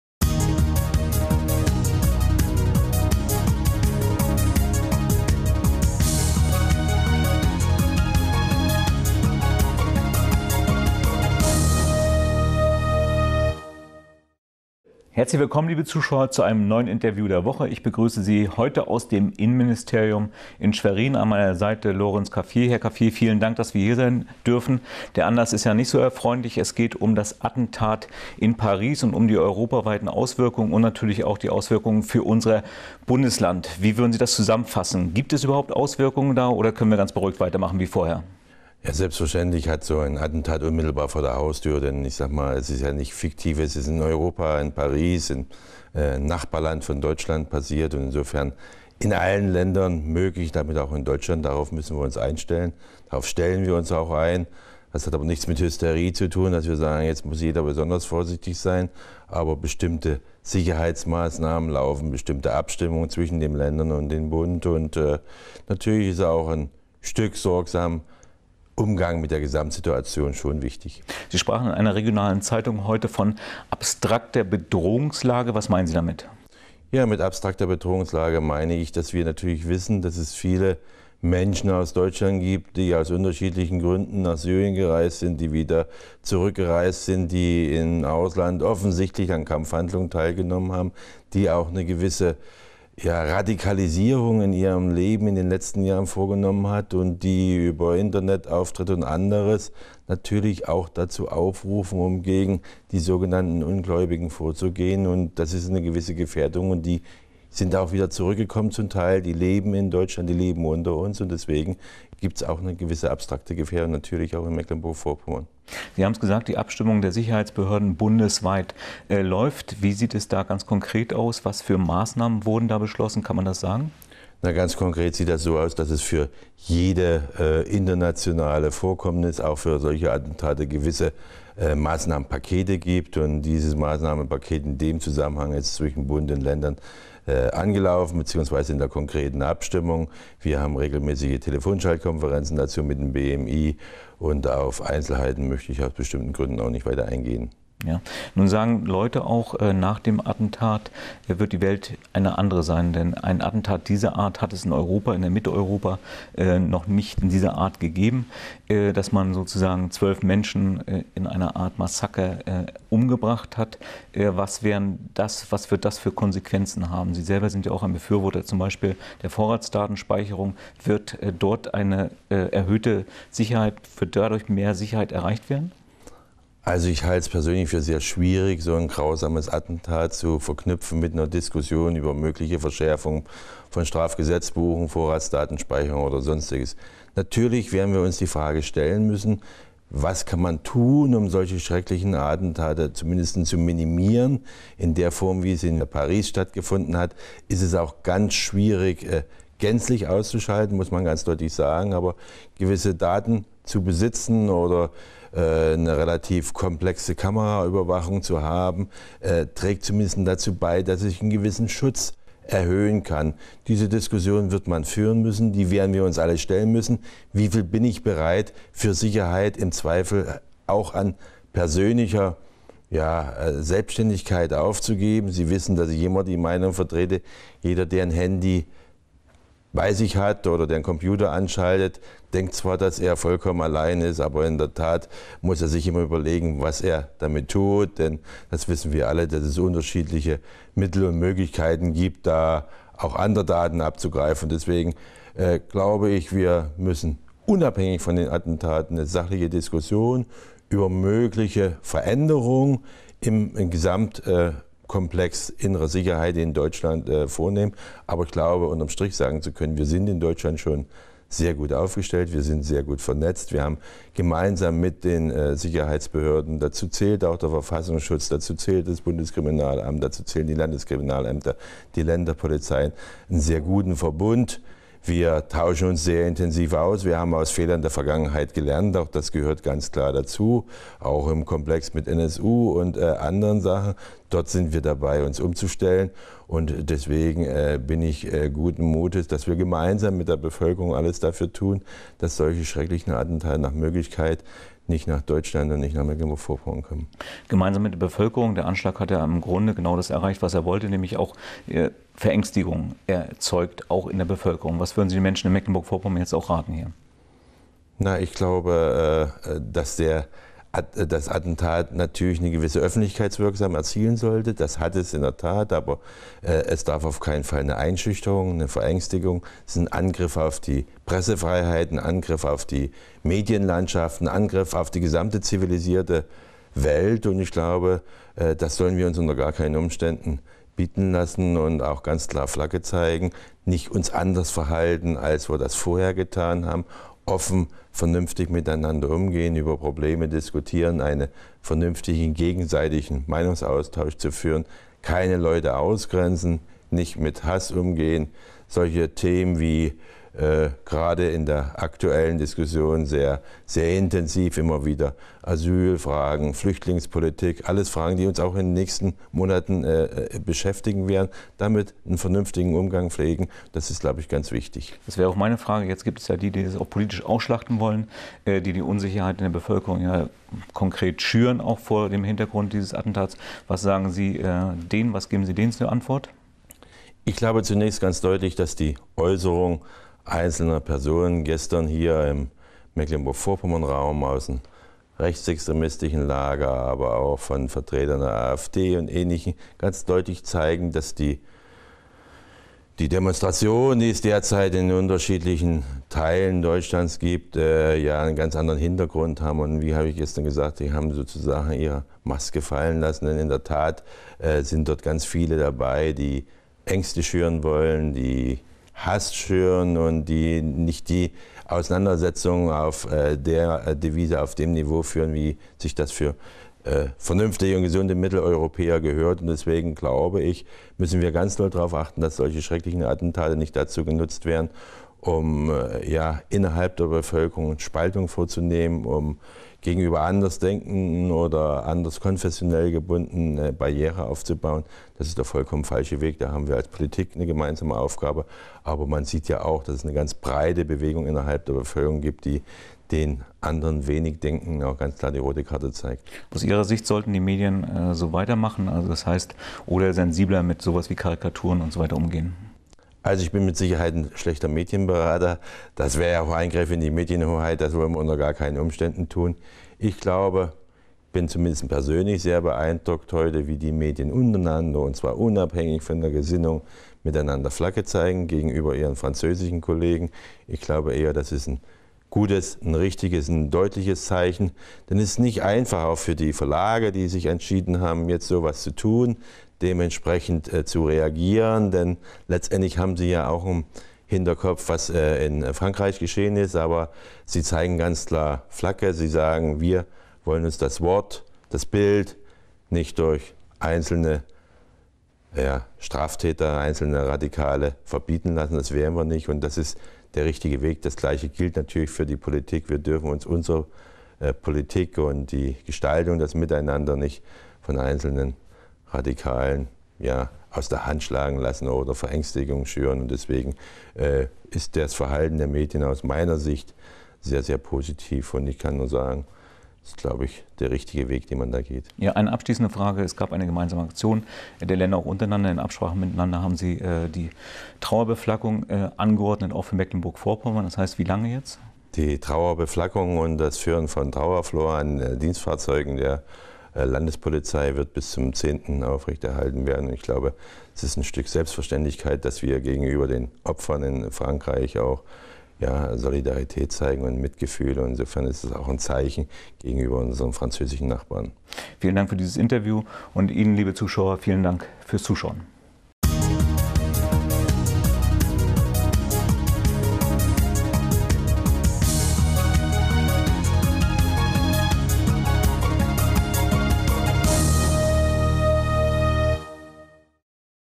Lorenz Caffier, Innenminister MV, zu den Anschlägen von Paris
Interview der Woche: Thomas Böhm, aus aktuellem Anlass des Anschlags in Paris im Gespräch mit dem Innenminister MVs Lorenz Caffier.